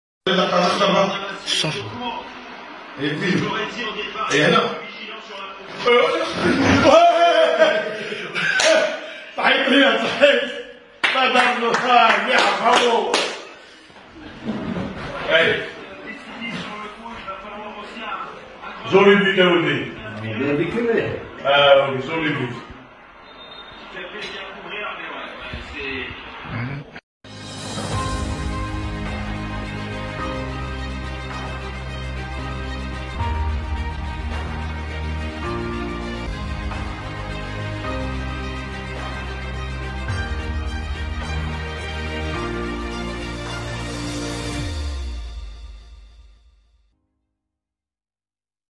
شاهد فرحة النجم لخضر بلومي بعد تسجيل محرز هدفه الأول ضد البياسجي
بلومي يفرج بتألق رياض محرز ضد البياسجي وفرحته بعد تسجيل محرز الهدف الأول